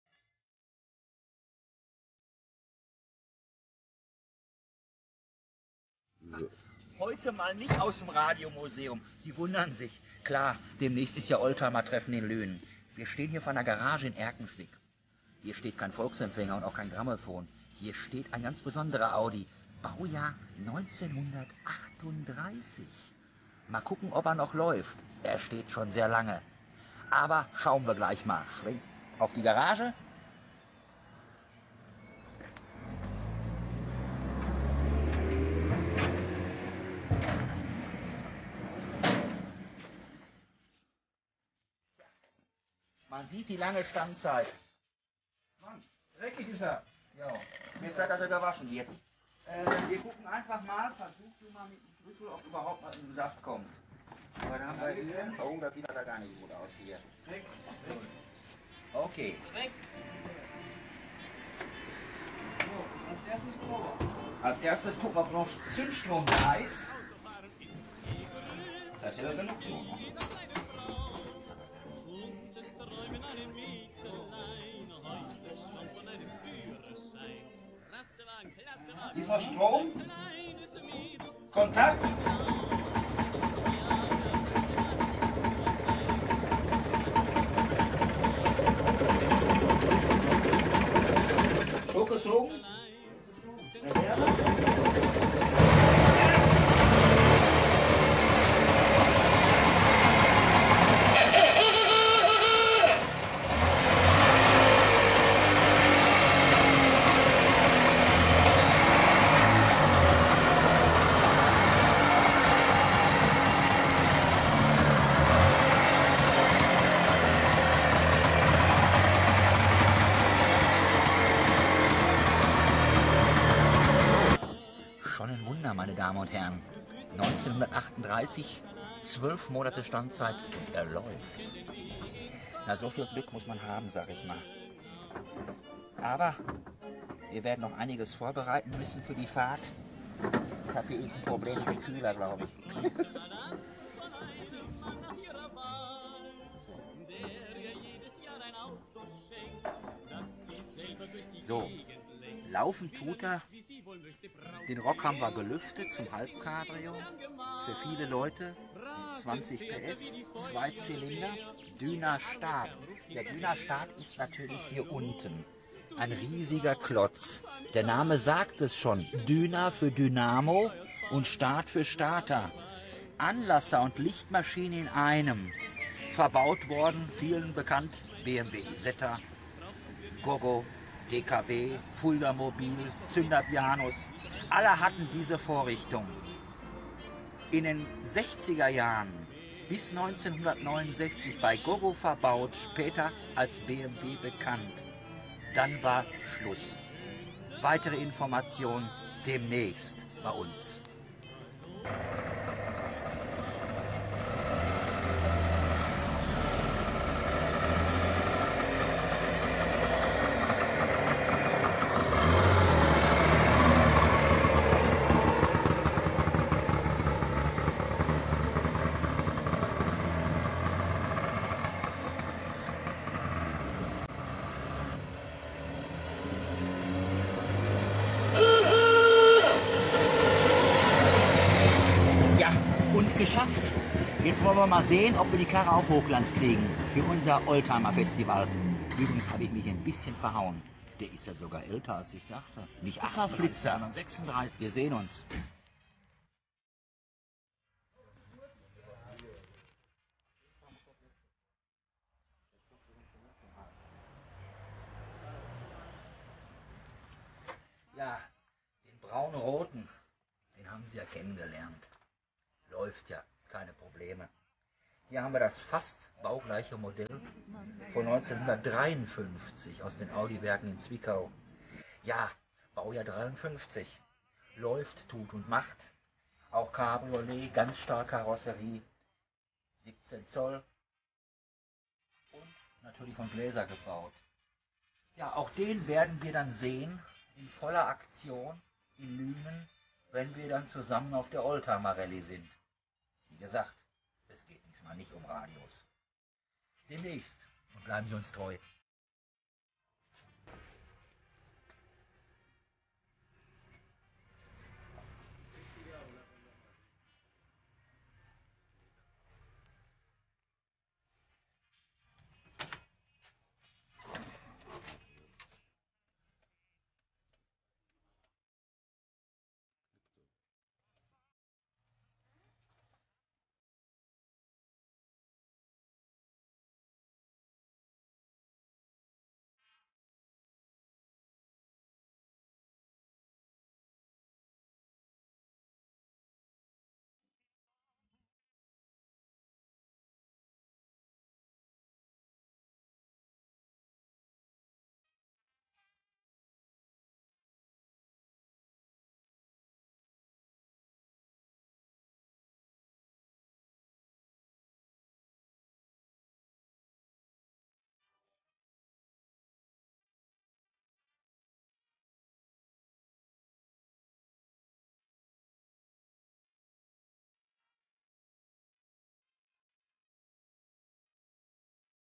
Radio Rainer auf Tour mit DKW
Oldtimer Treff in Lünen am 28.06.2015